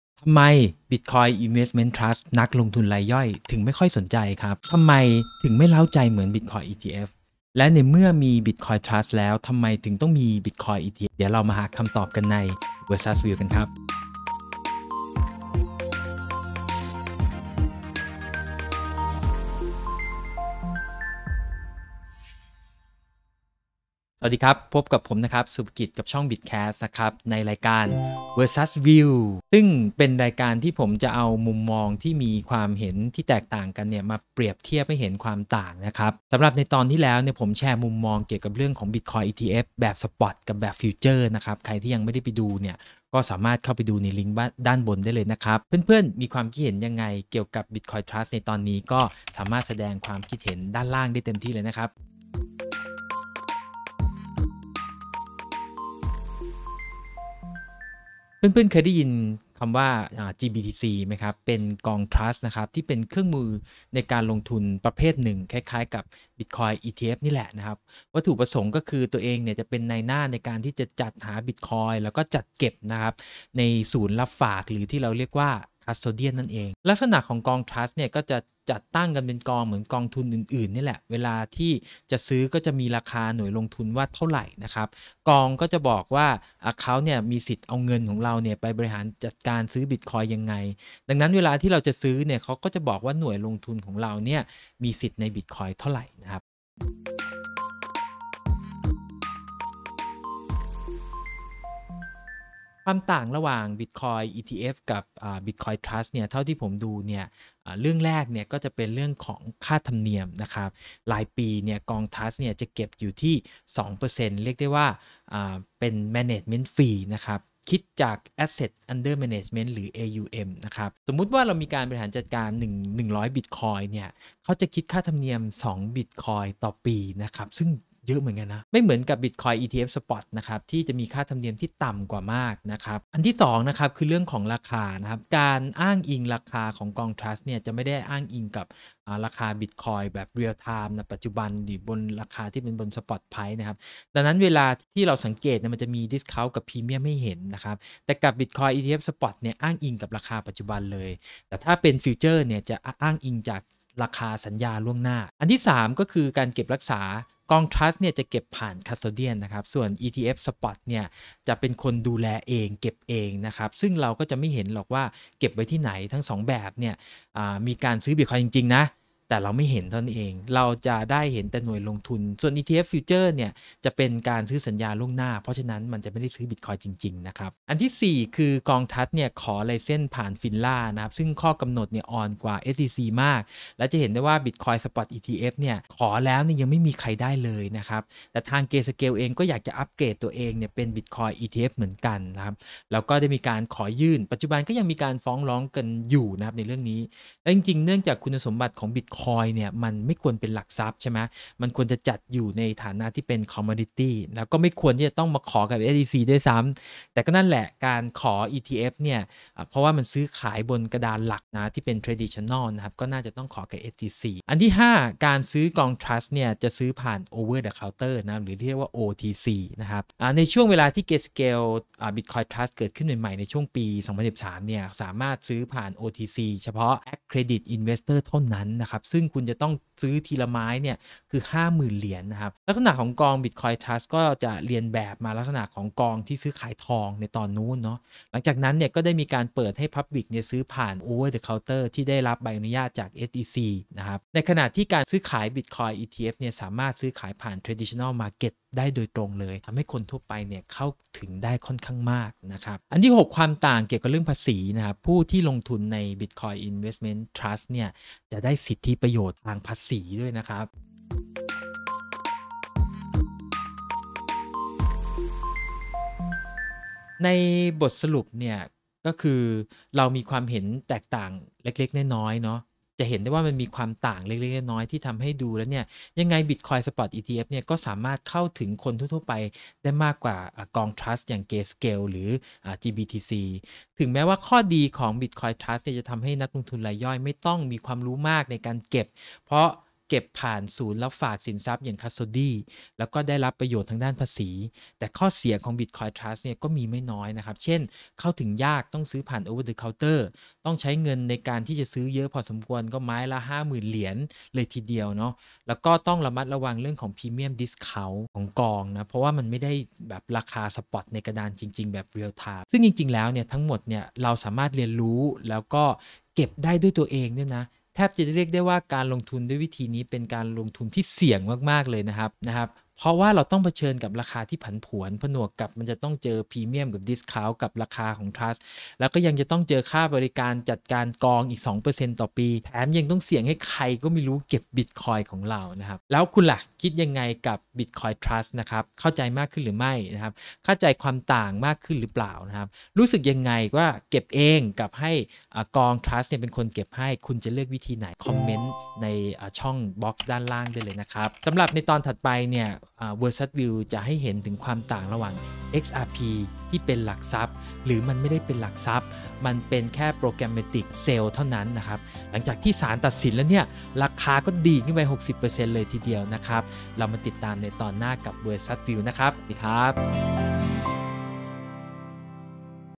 VersusView EP3: Bitcoin ETFs vs Bitcoin Trust ทำไมมี Bitcoin Trust แล้วยังต้องมี Bitcoin ETFs อีก
Description:
ทำไมมี Bitcoin Trust แล้วยังต้องมี Bitcoin ETFs อีก
เพื่อน ๆ เคยสงสัยไหมครับว่าทำไม่ Bitcoin Trust อย่าง GBTC. มีมาตั้งนานแล้ว ทำไมถึงไม่มีผลกับราคา Bitcoin มากขนาด  BitcoinETFs อะไรเป็นเหตุ เป็นปัจจัยให้เกิดผลแบบนั้นมาติดตามชมกันได้ใน VersusView กันครับ

00:00 อารัมภบท
00:24 บทนำ
00:52 Bitcoin Trust คืออะไร ?
01:38 ความต่างระหว่าง Bitcoin Trust กับ ETFs
04:23 บทสรุป Bitcoin Trust กับ ETFs

คอร์สอบรมกระเป๋าคริปโตสำหรับมือใหม่
เยี่ยมชมเว็บไซต์ 👉👉👉
ท่านสามารถร่วม Join Membership ได้แล้ววันนี้ ขอบคุณทุกท่านที่สนับสนุนช่อง Bitcast